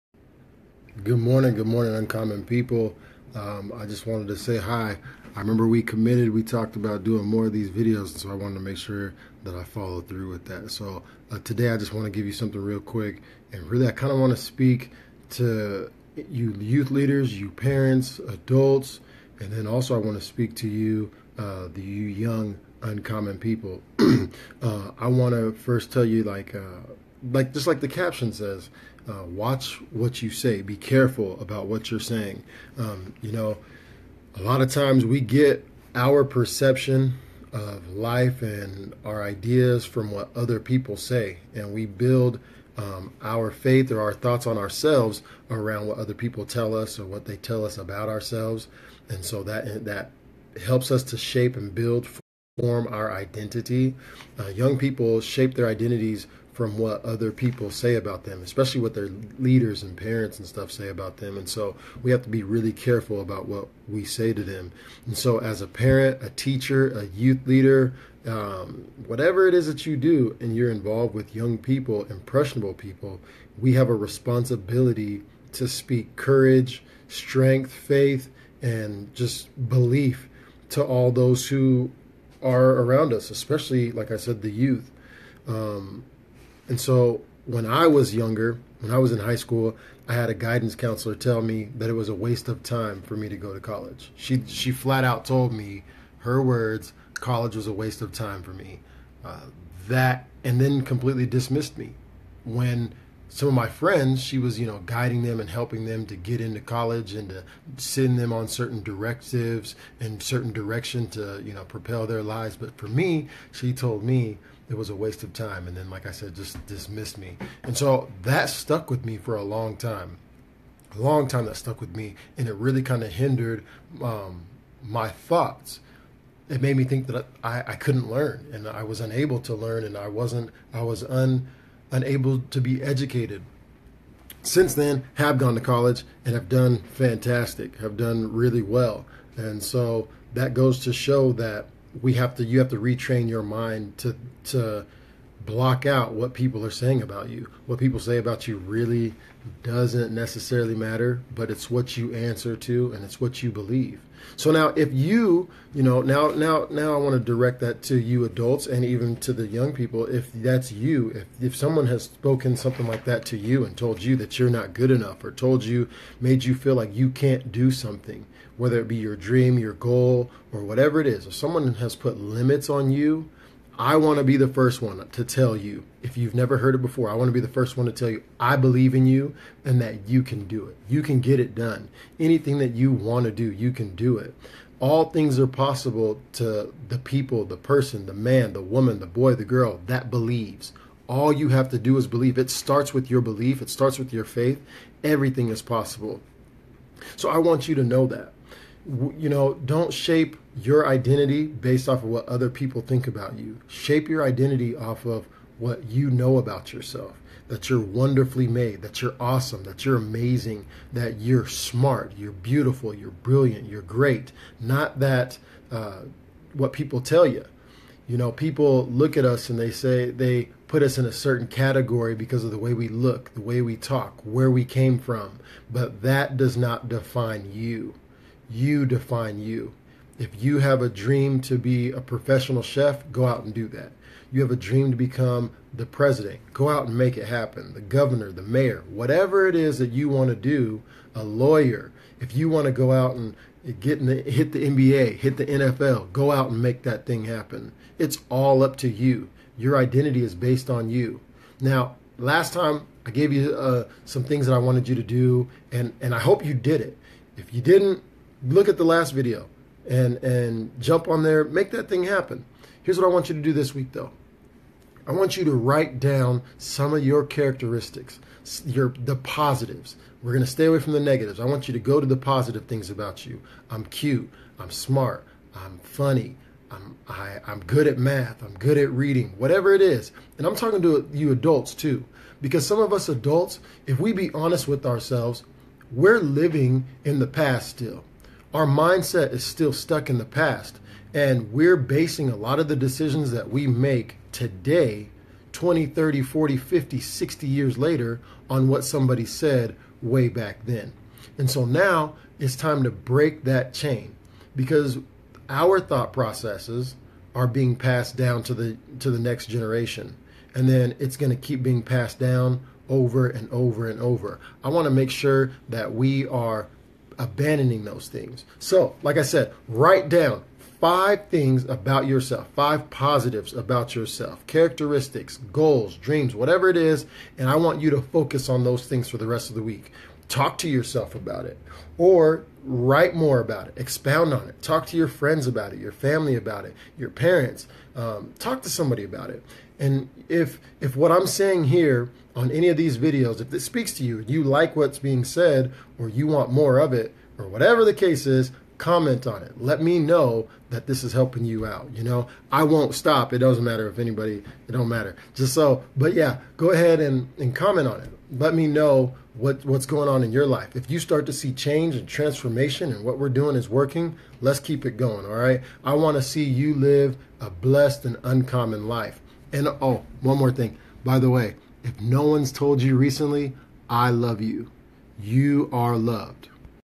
Uncommon People | Careful what you say!
Our word carry power. The words we speak carry power and shape the world around us.